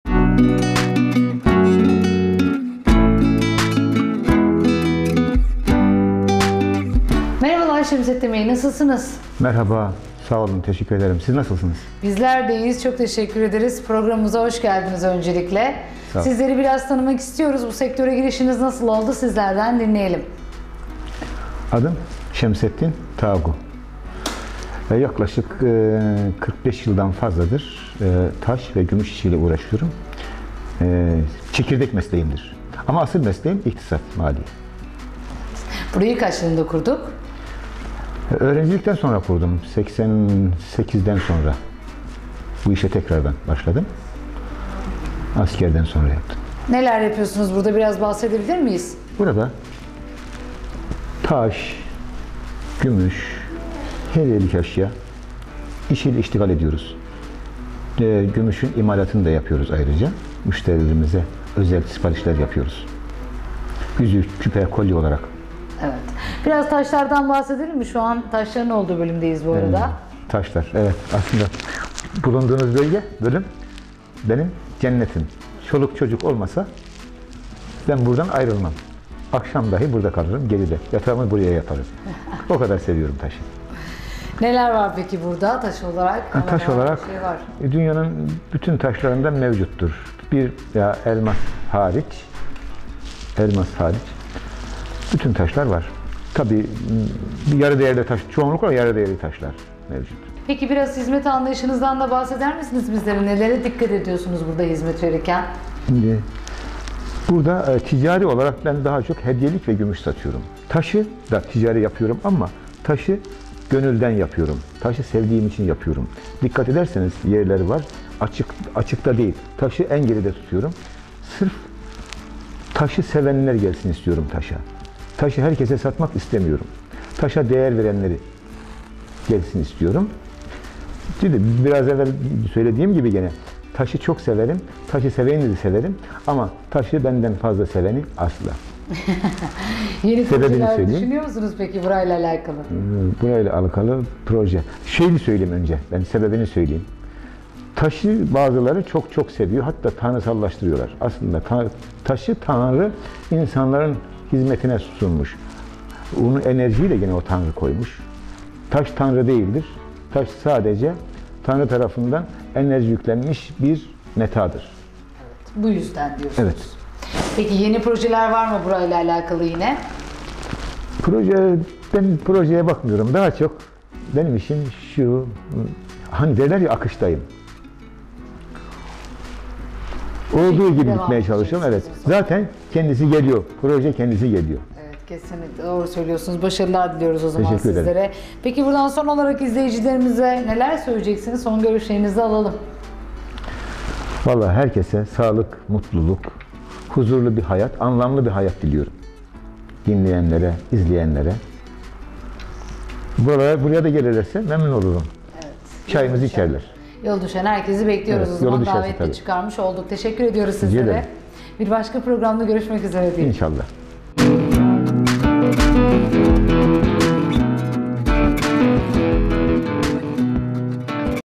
ÜSTÜN HİZMET |TAGU GÜMÜŞ
Bursa Osmangazi’de faaliyetlerine devam eden ‘’Tagu Gümüş” firma sahibi Şemsettin Tagu, Posta Kobi'ye sektörel başarılarını anlattı.